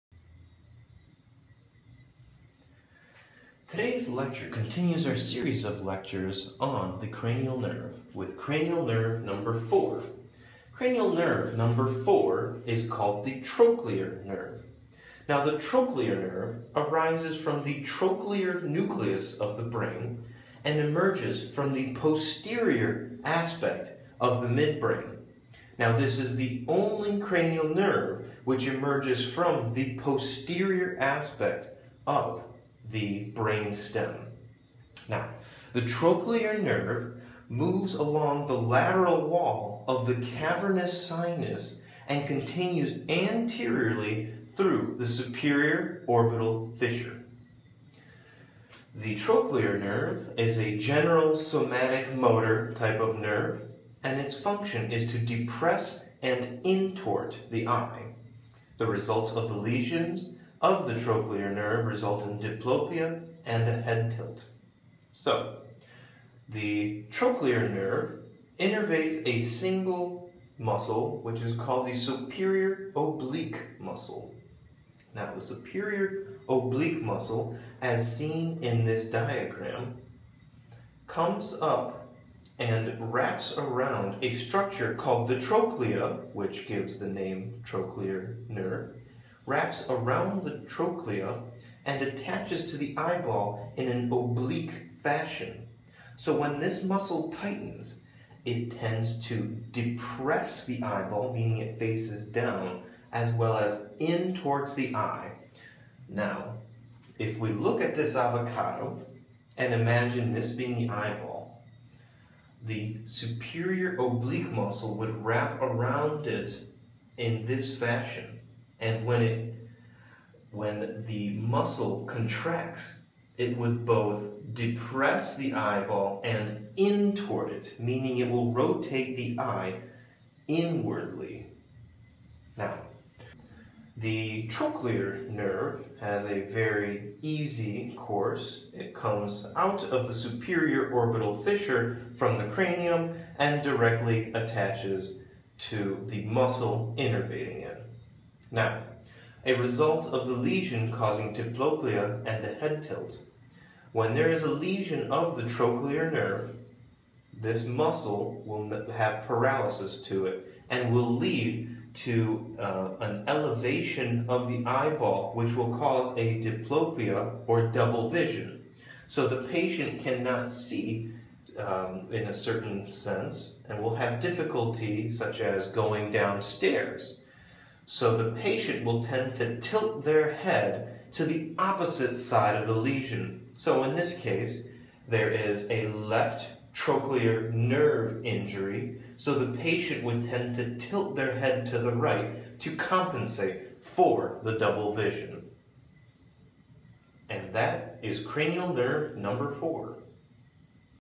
Cranial Nerve IV - Anatomy Lecture for Medical Students - USMLE Step 1
Cranial Nerve IV Simply Explained. Anatomy lecture for Medical Students BY medical students studying for the USMLE Step 1 or NCLEX